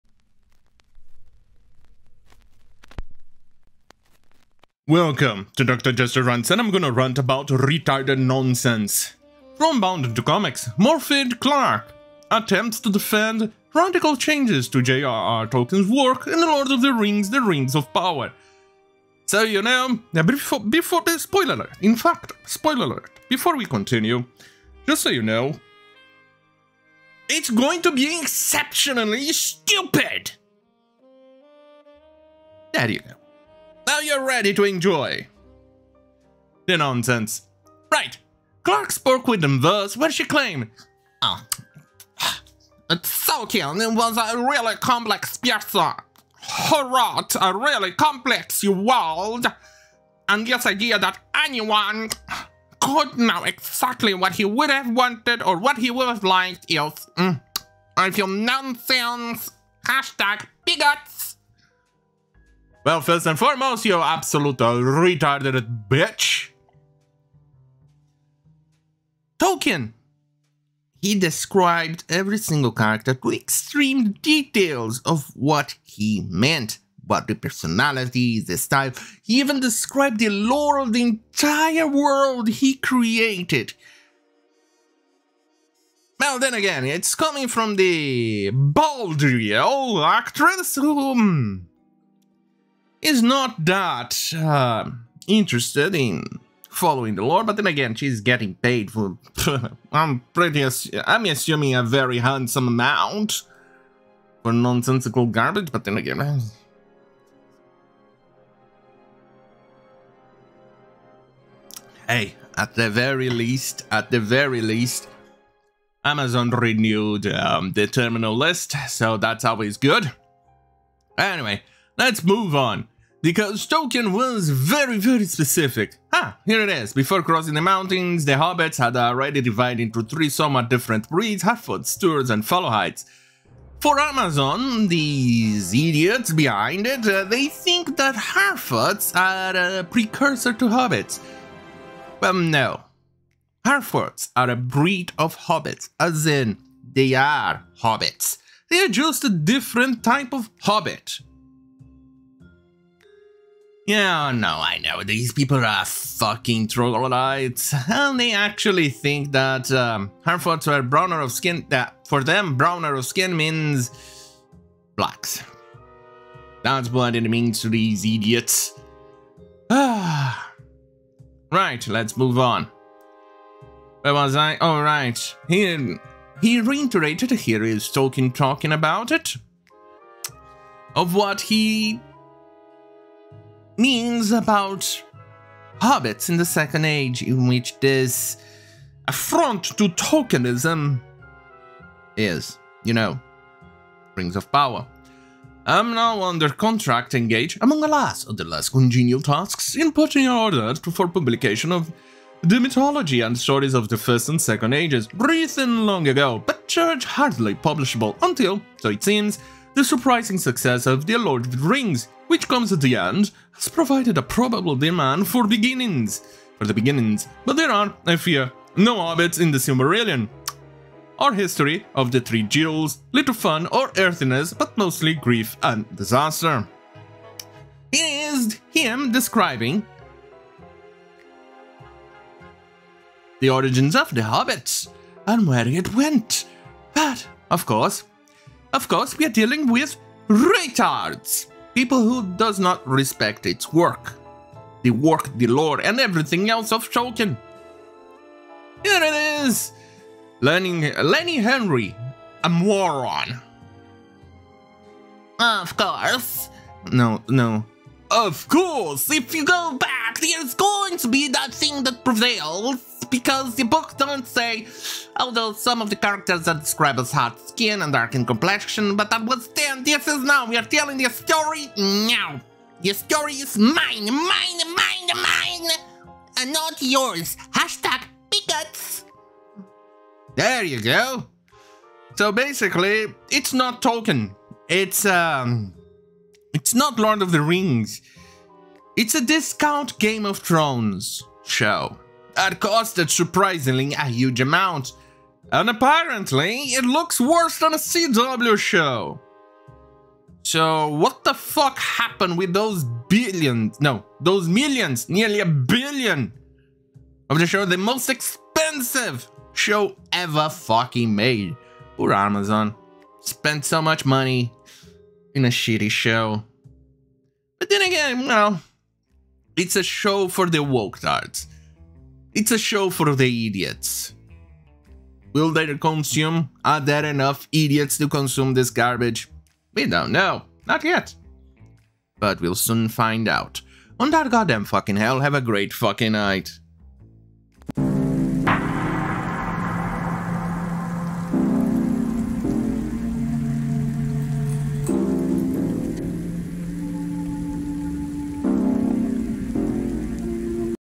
Balldriel Actress Morfydd Clark Does A Shoddy Job In Defending The Changes In Rings Of Power
PS: Youtube seems to be deleting comments randomly. Even innocent comments that doesn't even come close in breaking any TOS. *Honk* *Honk*

Sounds:
Horseyfootage - Creative Commons 3.0 - Suspense Echo Hit
Mark DiAngelo - Creative Commons 3.0 - Rain Sound